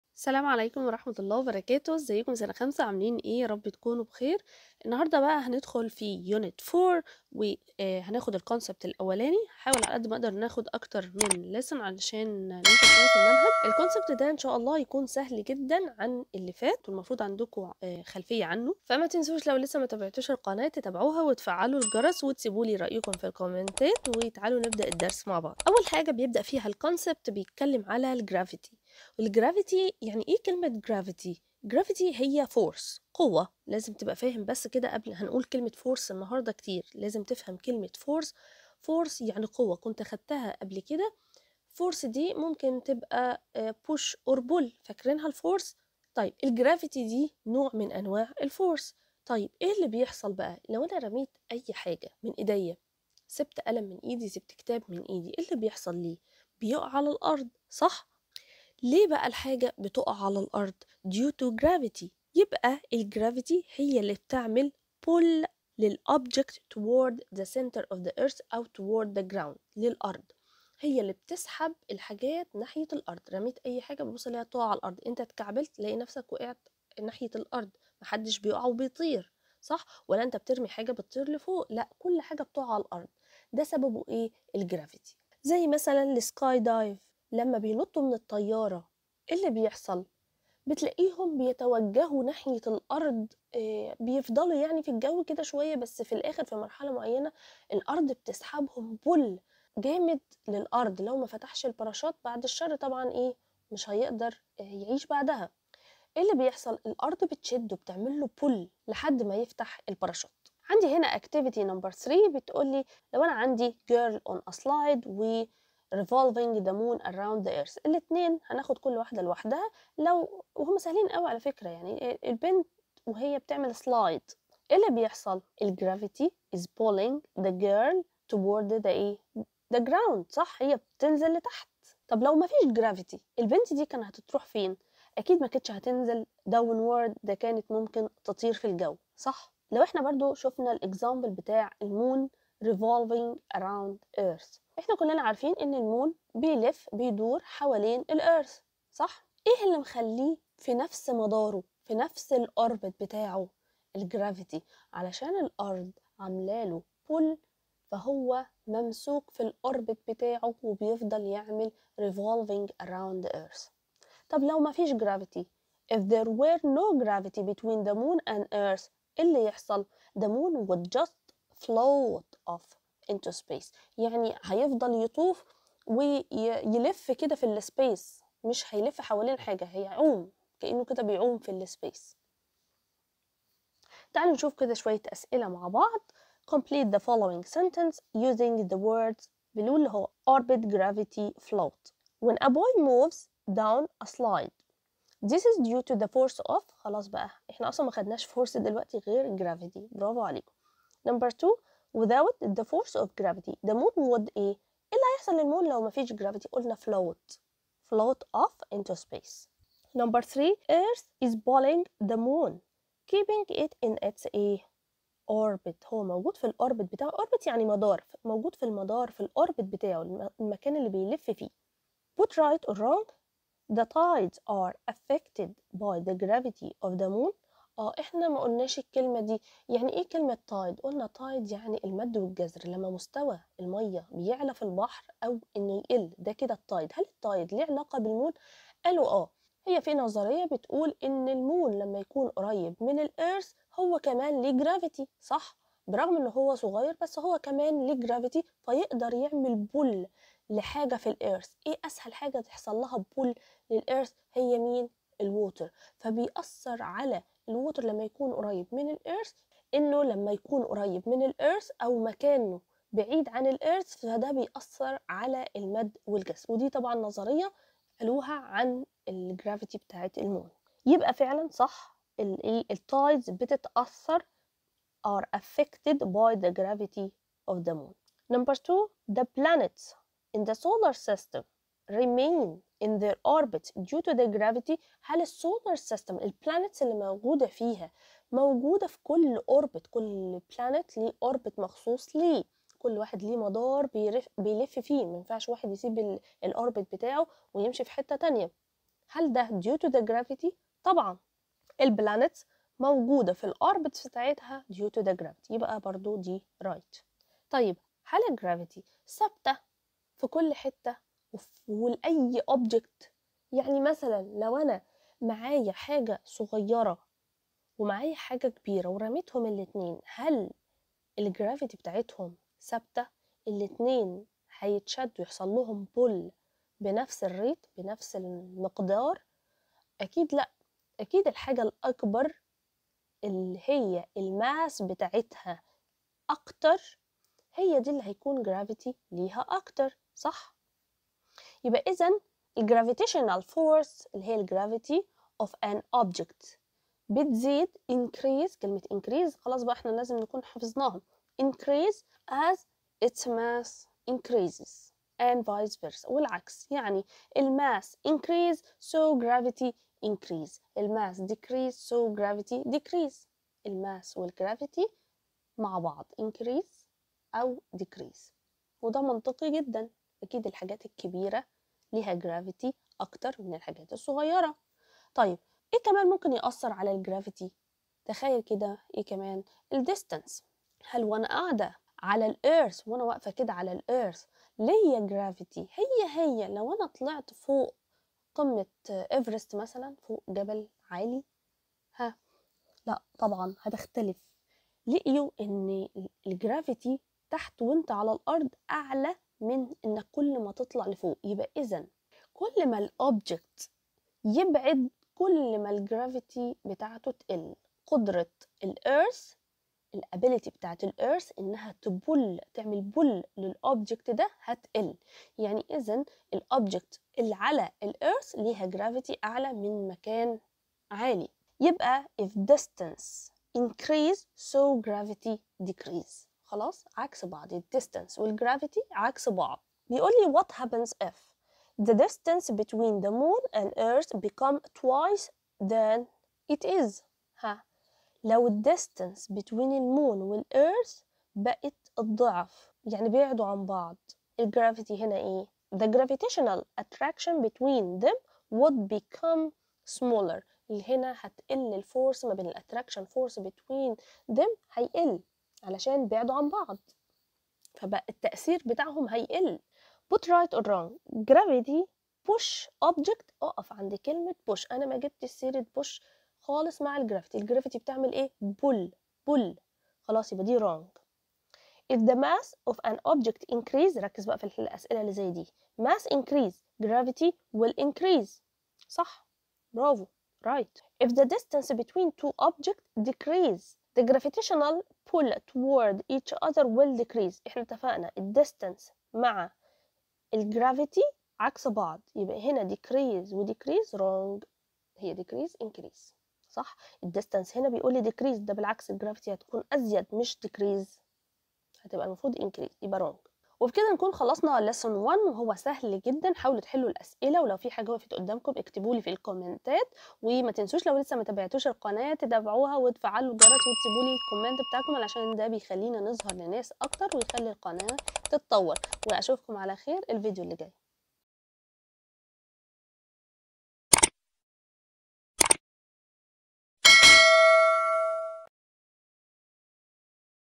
Science Grade 5 - Concept 4.1 - Lesson 1
المنهج الجديد 2023 - شرح مادة العلوم لمدارس اللغات الصف الخامس الإبتدائي
اصدقاء كتير بيتوهو وسط الكتب والمناهج .. وشايفين ان العلوم (الساينس) مادة مرعبة .. هانحاول نبسطلكم المنهج علشان تذاكرو لاولادكم ويحبو الساينس .. وماحدش هايذاكر لابنك احسن منك .. تابعوا سلسلة من الفيديوهات لشرح الساينس بكل سهولة